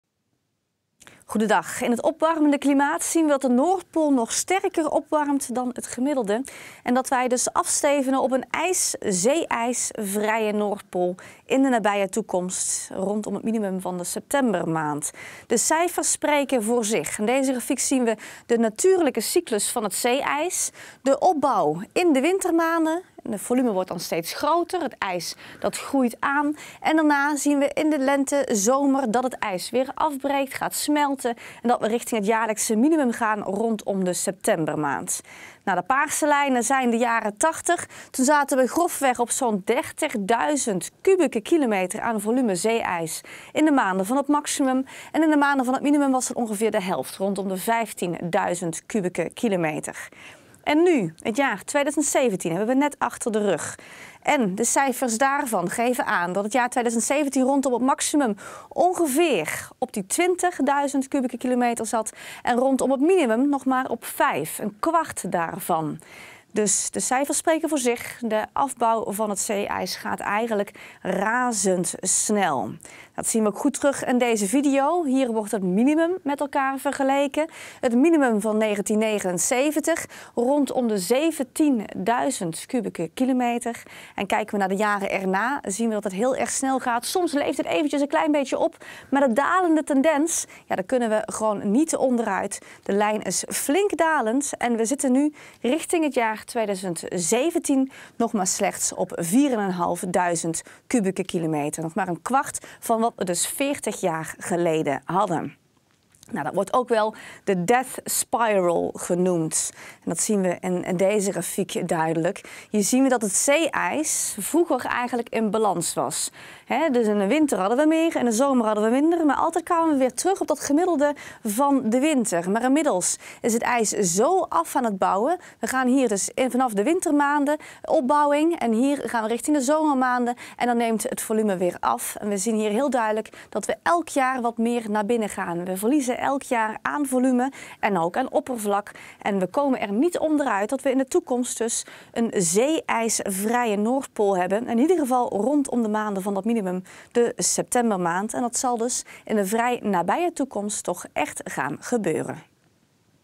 The Death Spiral van het zeeijs op de Noordpool
Het is onvermijdelijk, in de nabije toekomst hebben we een zeeijsvrije Noordpool (in september). Meer informatie op